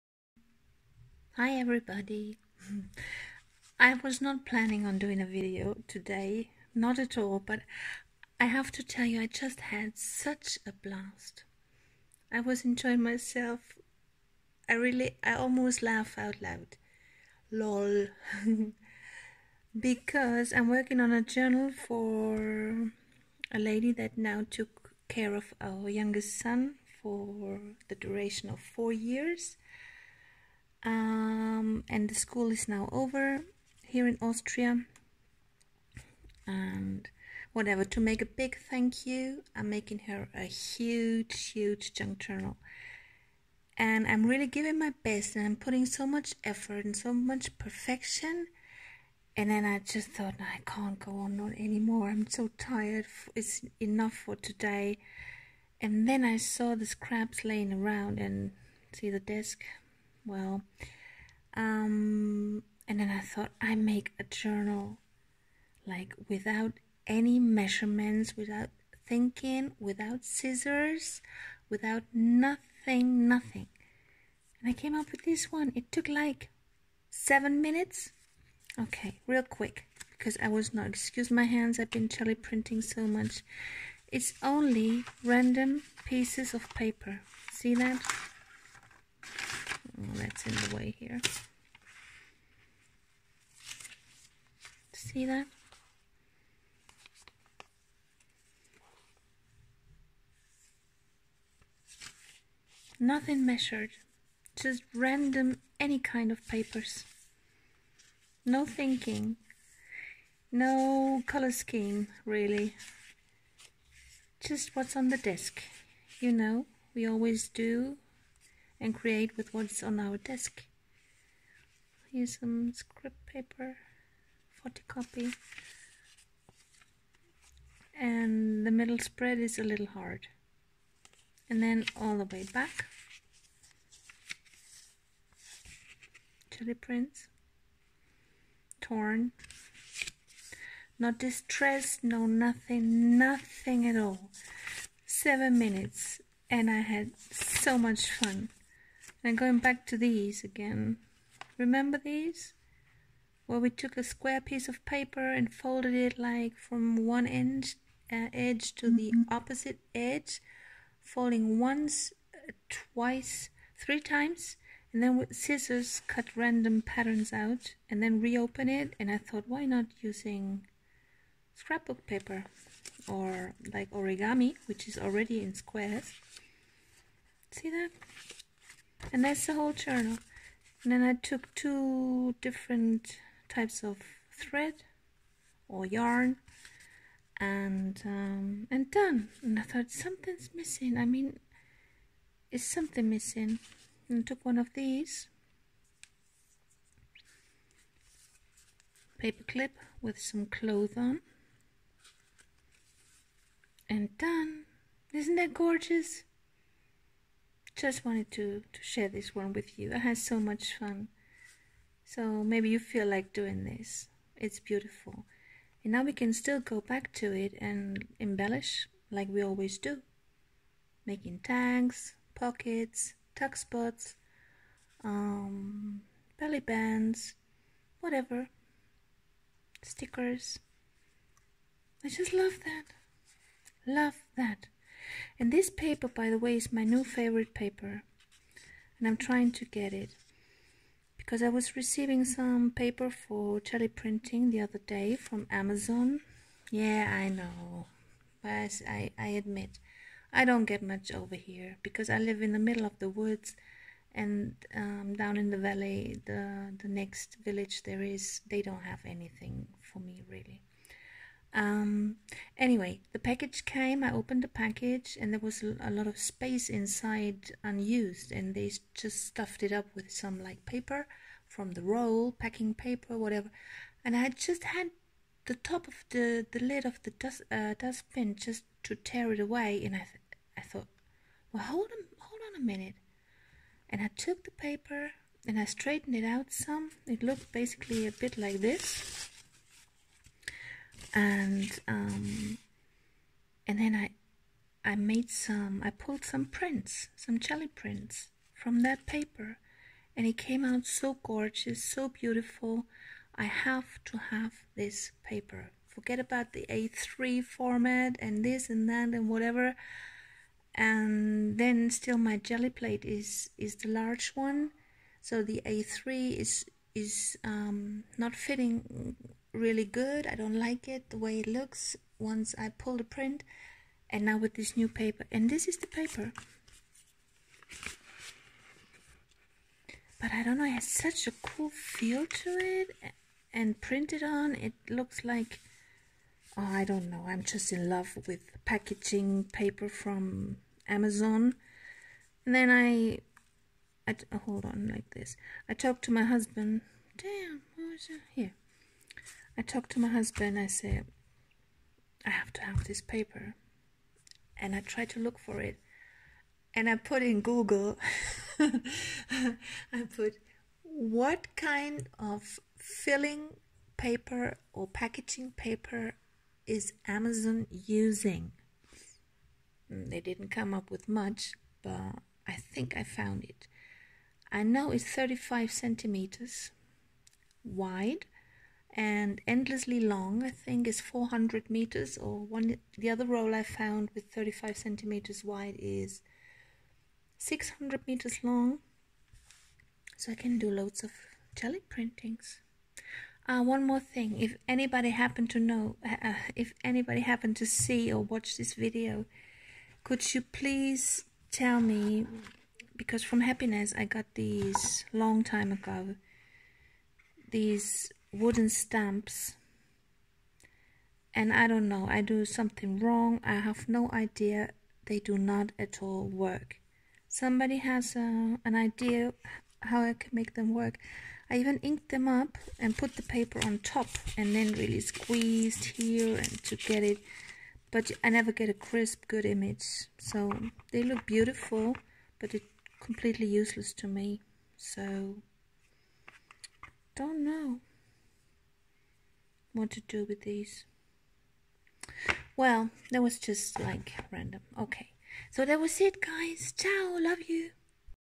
#10 Crazy 7 min junky junk journal as insert in a "real" journal. What a blast I had!
Mini journal without supplies, glue, tools. just going crazy and wild and having a real good, non-perfection time. Try it out, it's so liberating.